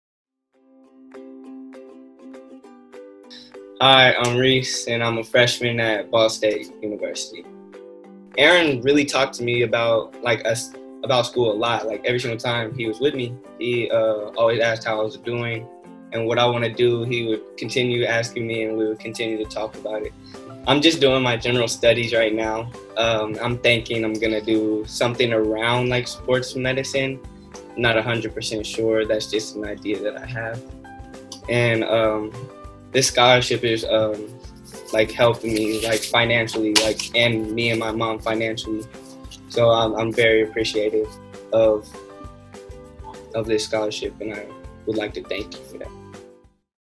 Reis – 2020 BBBSCI Graduate and Recipient of the Strada Big Potential Scholarship
Meet Reis, a 2020 graduate of the BBBSCI program and freshmen at Ball State University. Each year, Strada Education Network partners with Big Brothers Big Sisters of Central Indiana to award a graduating Little Brother or Sister with the Big Potential Scholarship. This multi-year scholarship supports Big Brothers Big Sisters of Central Indiana’s efforts to further defend the potential of our community’s youth.